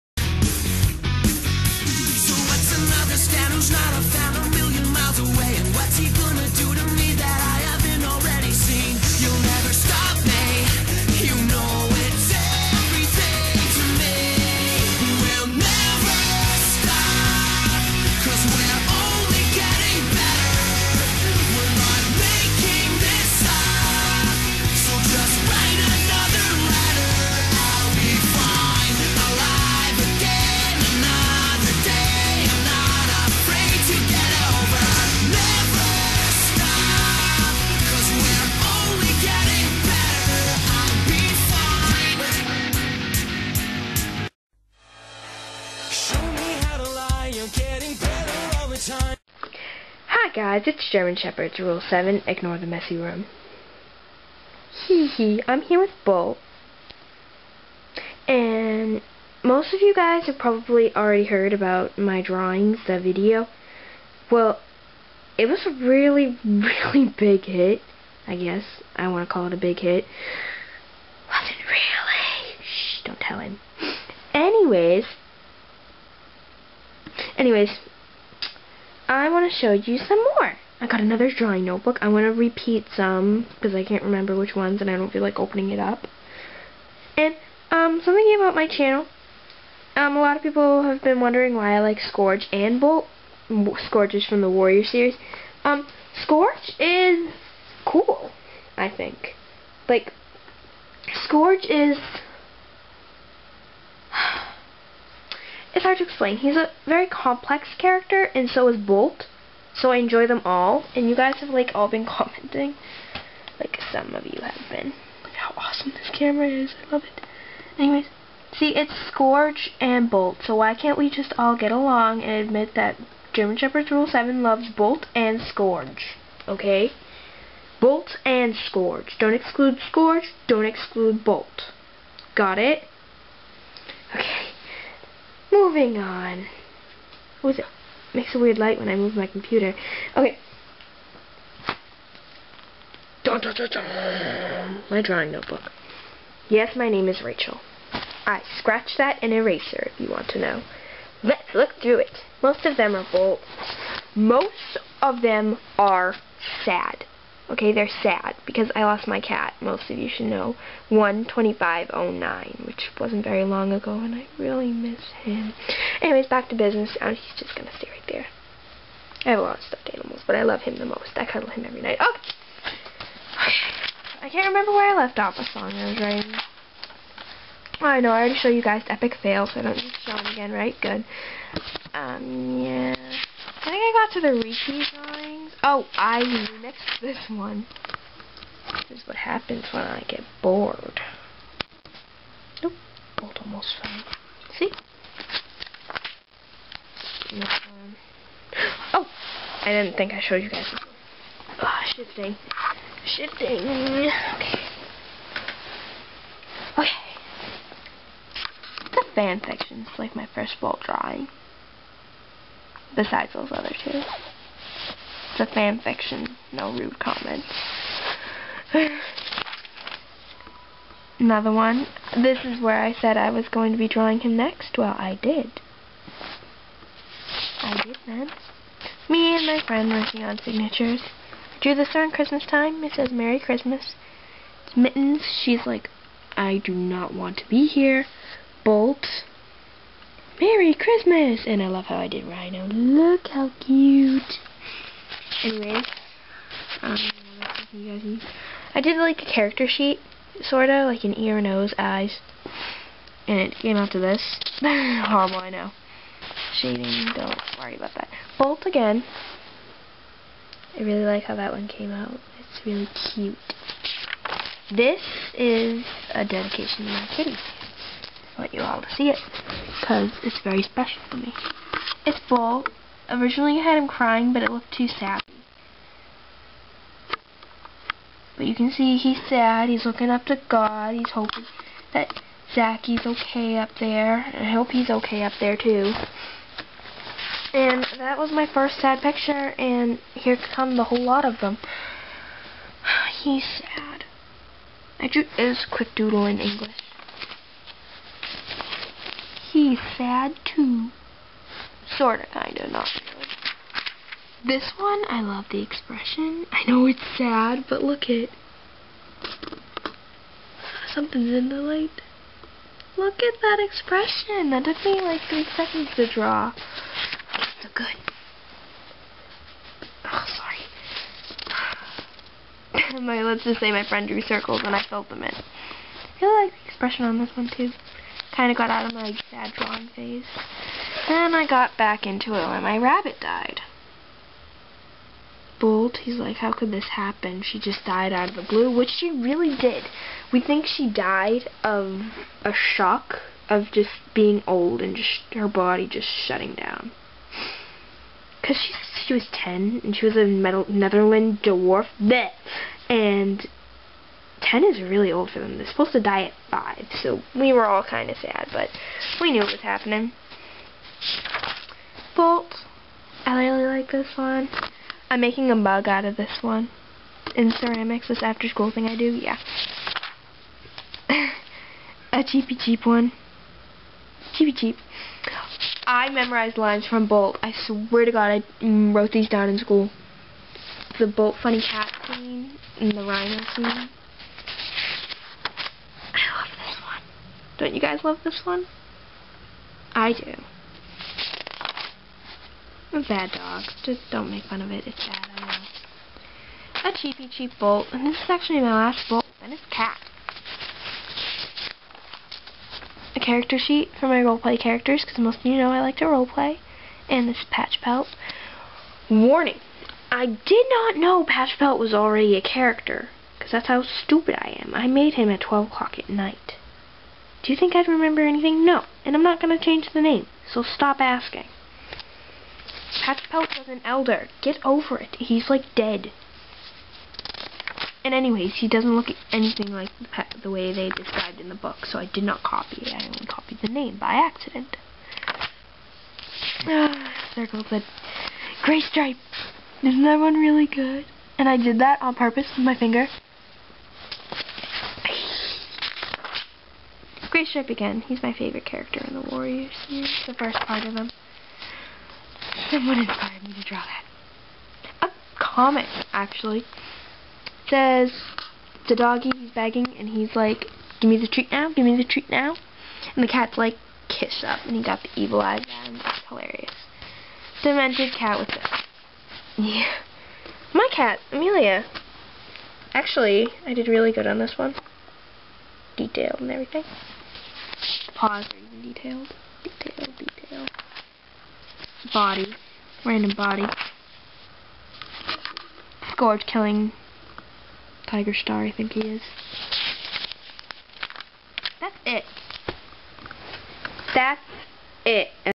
Some More Of My Drawings!!
I AM NOW CALLED THUNDERBOLT PRODUCTIONS!!!

Ok well its basically what the video says just an update on my drawings and other stuff ok now i know what your thinking at the place where i said i memorized some lines from the movie Bolt WRONG! I memorized all i do the movie with my plushies! ((i am such a nerd)) anyways i really can recite the whole movie dont belevie me i would make a video but it would be to long and this is the beggining to a new series on GERMANSHEPARDSRULE7
it called the BOLT&MITTENS Show!!! me and my sister are gonna do funny skits with MY Plushies lol she doesnt have a yt so dont ask and someone ((not saying who)) said that alot of my vids have been removed or muted u wanna see them then go there you will fing half of my videos!